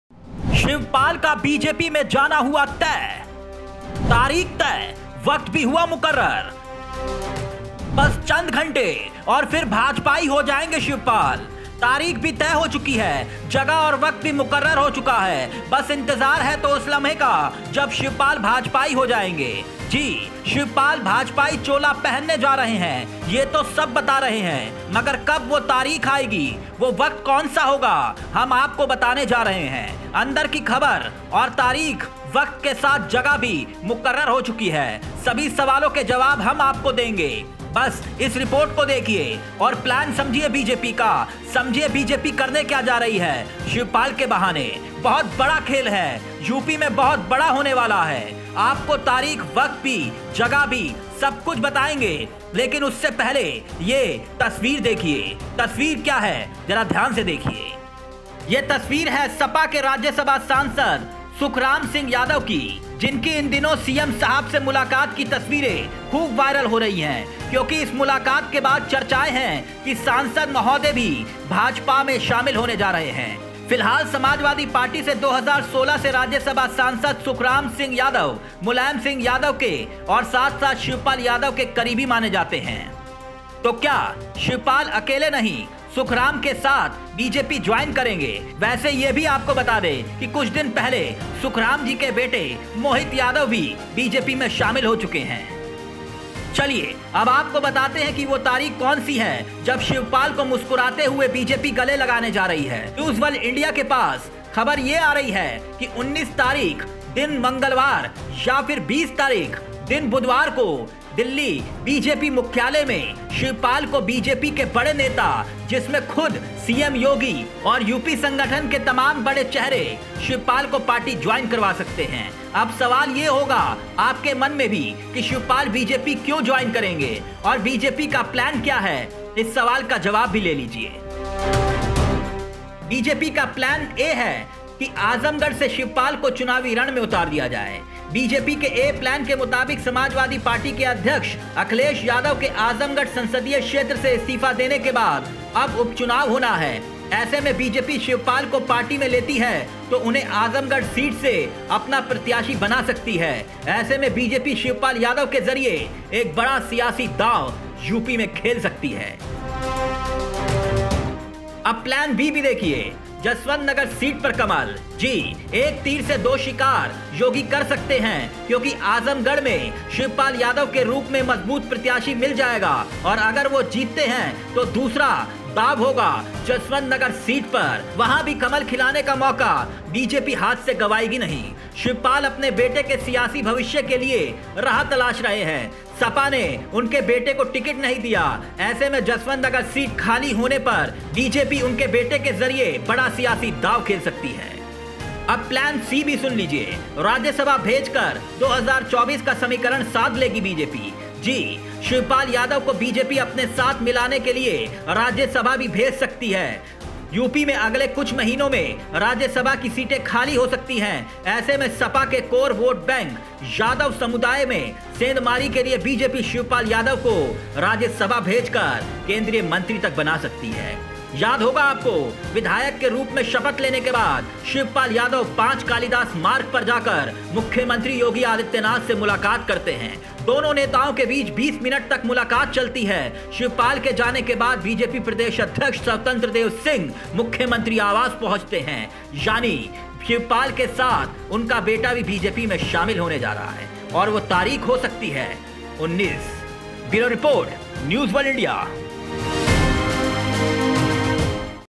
Shivpal yadav को लेकर बड़ा खुलासा ! इस दिन BJP में होंगे शामिल ?
plan A
बीजेपी आजमगढ़ से शिवपाल को उतार सकती है
बीजेपी के planA के मुताबिक सपा अध्यक्ष अखिलेश यादव के आजमगढ़ संसदीय क्षेत्र से इस्तीफा देने के बाद अब उपचुनाव होना है. ऐसे बीजेपी शिवपाल को पार्टी में लेती है तो उन्हें आजमगढ़ सीट से अपना प्रत्याशी बना सकती है. ऐसे में बीजेपी शिवपाल यादव के जरिए बड़ा सियासी दांव खेल सकती है.
plan B
 जसवंतनगर सीट पर कमल !
एक तीर से दो शिकार योगी कर सकते हैं क्योंकि आजमगढ़ में शिवपाल यादव के रूप में मजबूत प्रत्याशी मिल जाएगा और अगर वो जीतते हैं तो दूसरा जसवंतनगर सीट पर भी कमल खिलाने का मौका मिल जाएगा. शिवपाल अपने बेट के सियासी भविष्य के लिए राह तलाश रहे हैं. सपा ने उनके बेटे को टिकट नहीं दिया था. ऐसे में जसवंतनगर सीट खाली होने पर बीजेपी उनके बेटे के जरिए बड़ा सियासी दांव चल सकती है. योगी के पास एक दांव और भी है और वो दांव है ये
plan c
राज्यसभा भेजकर 2024 का समीकरण साधेगी
शिवपाल यादव को बीजेपी अपने साथ मिलाने के लिए राज्यसभा भेजने का दांव भी चल सकती है. यूपी में अगले कुछ महीने में राज्यसभा की सीटें खाली हो सकती है. ऐसे में सपा के कोर वोटबैंक यादव समुदाय में सेंधमारी के लिए बीजेपी शिवपाल यादव को उच्चसदन भेजकर केंद्रीय मंत्री बनाने का दांव चल सकती है.